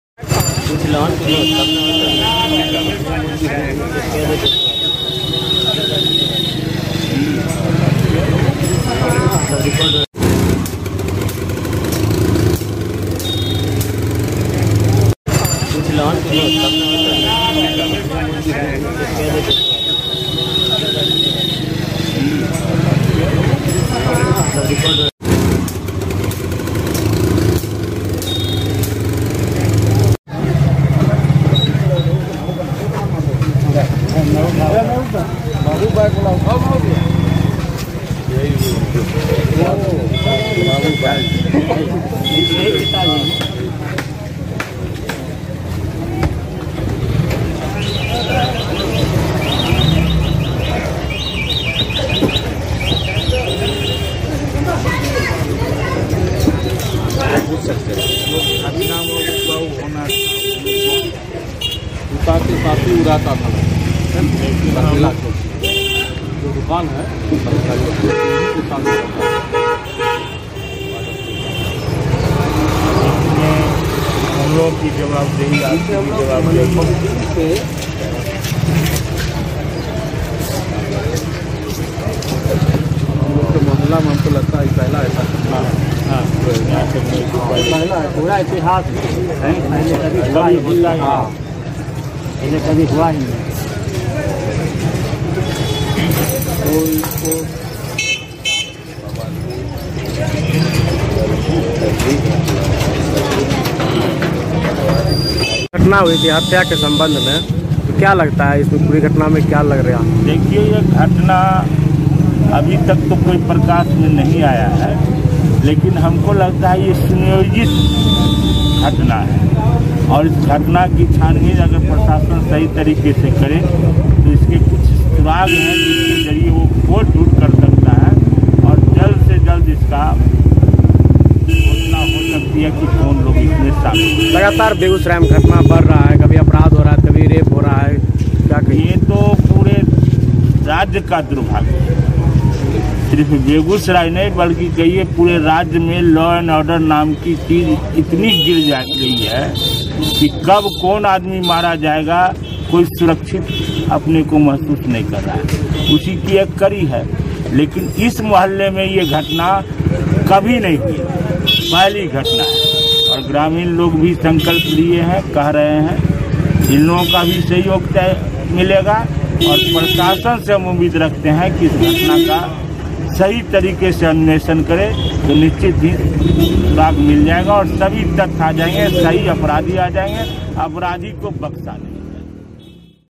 Begusarai : लड़के की हत्या के बाद पीड़ित परिवार से मिलने पहुंचे सीपीआई नेता ।
Begusarai : लड़के की हत्या के बाद पीड़ित परिवार से मिलने पहुंचे सीपीआई नेता।patnanews